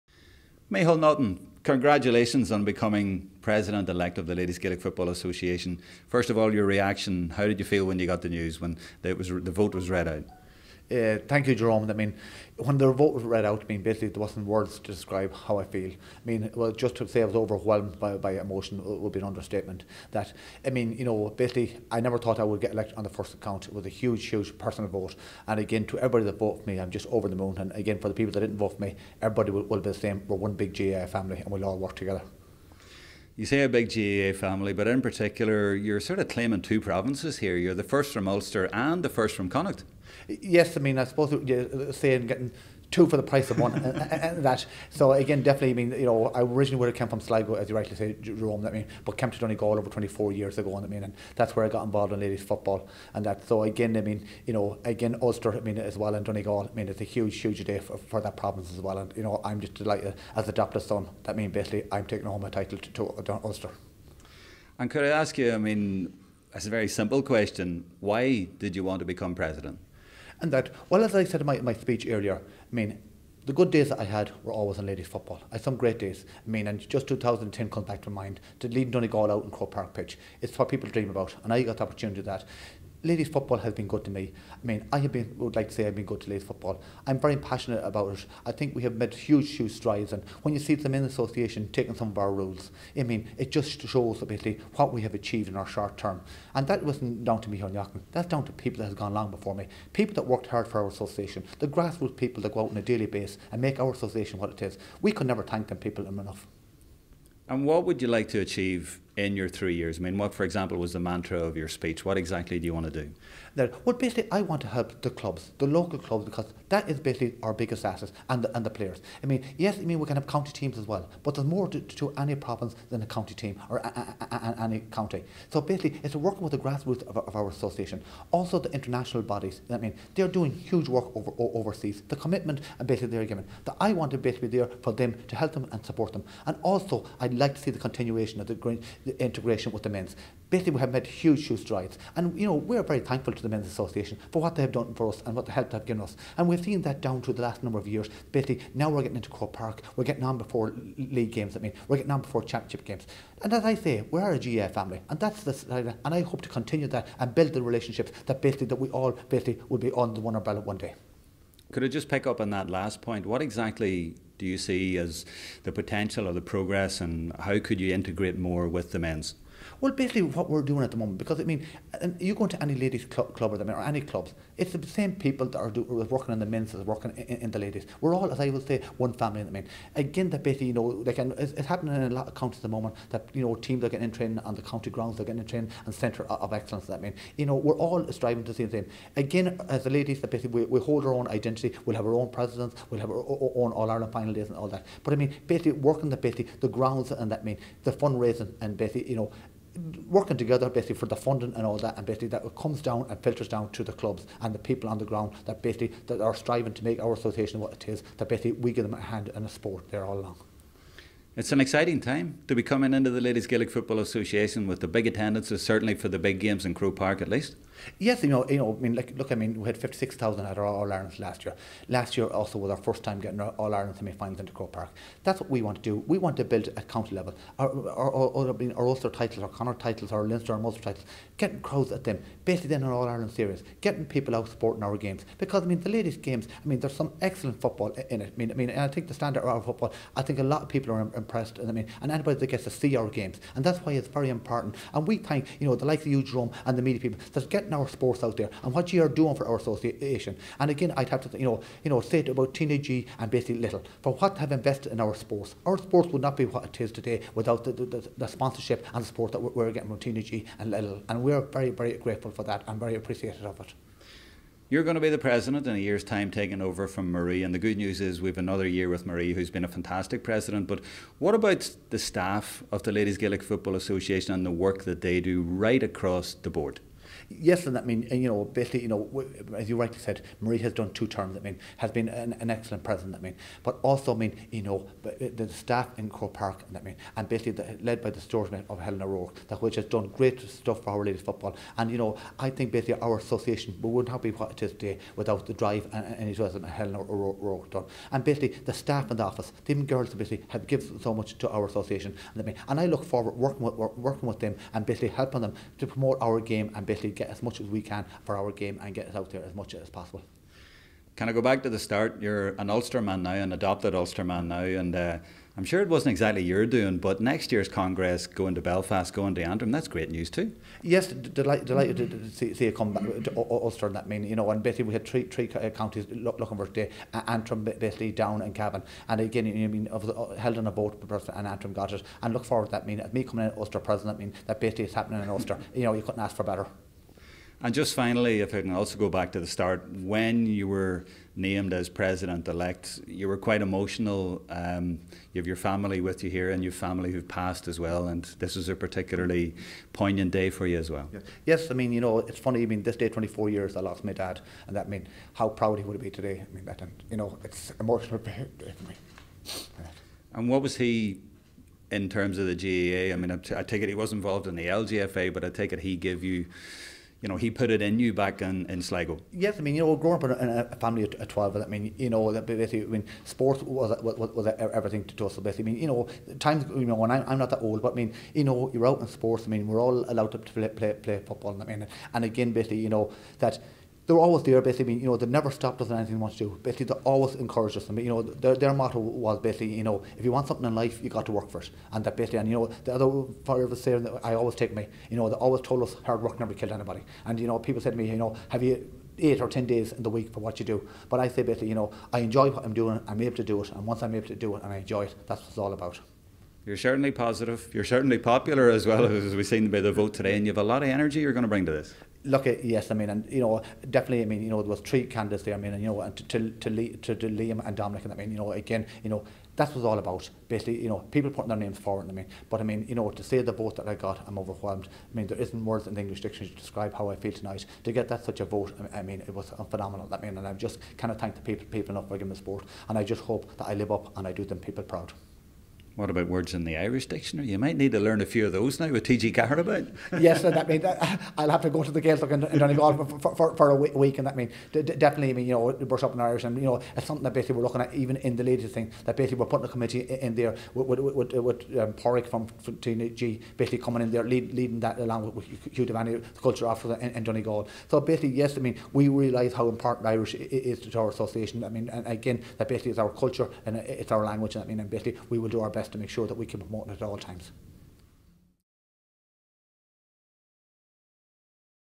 Extended Interview with Michael Naughton
'This day 24 years ago I lost my Dad. How proud he would be today' - see Jerome Quinn's first interview with new LGFA President-Elect Michael Naughton after the election at Congress in Galway.

The former Donegal manager, who spent his early years in a family of 12 in Sligo, outlines his hopes for the future of the organisation, with a focus on clubs, international units and integration.

‘The good days I had were all in Ladies Football’, says Naughton. ‘I’m very passionate about it, we have made huge strides and we want more people out at our games’.